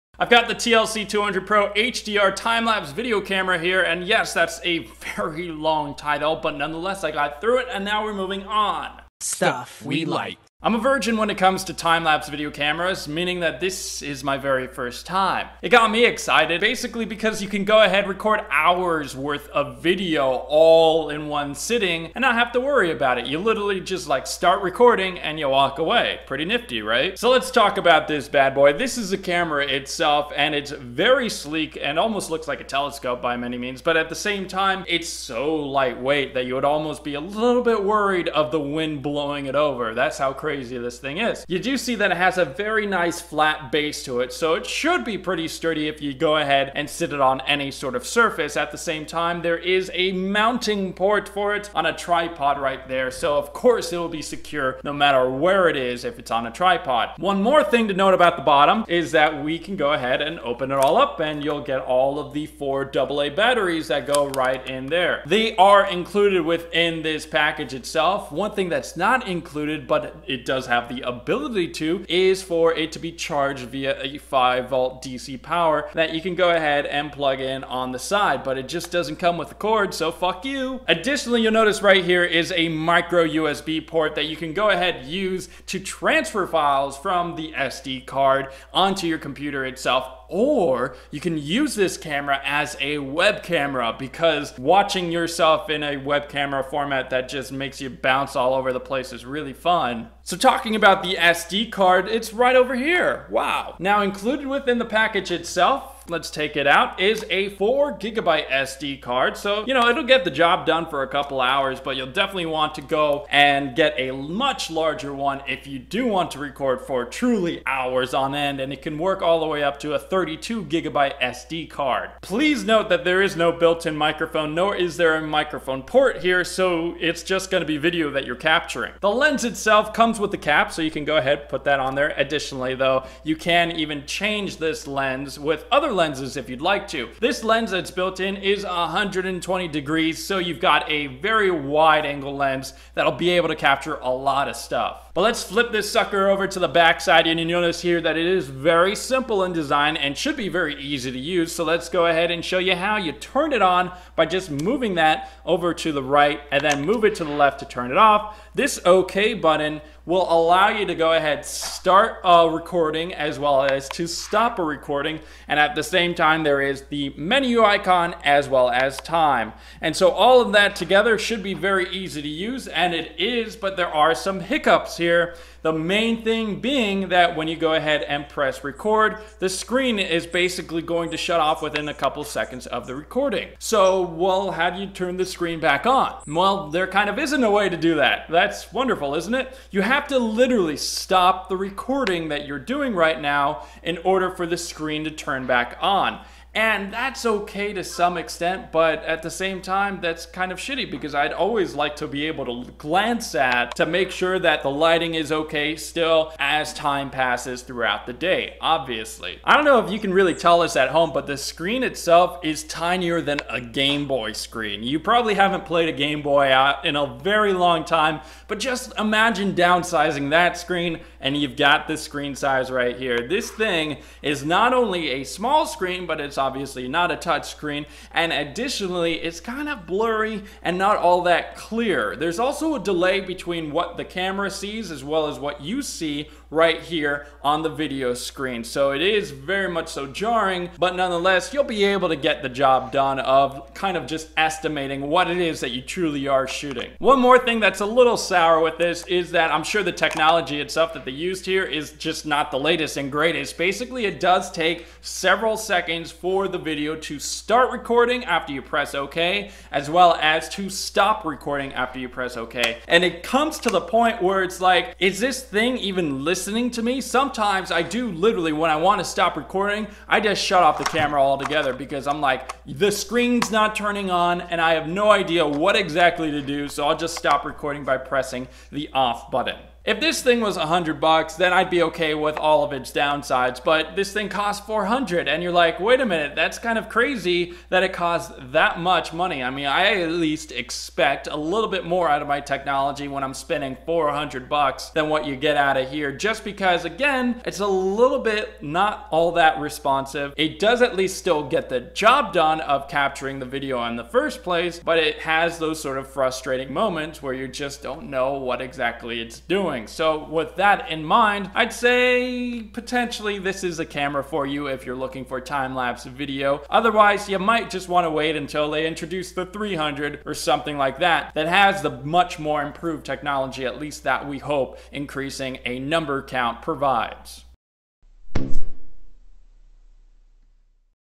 Dave Reviews TLC 200 Pro HDR Time Lapse Video Camera
Dave reviews the Brinno TLC 200 Pro HDR Time Lapse Video Camera.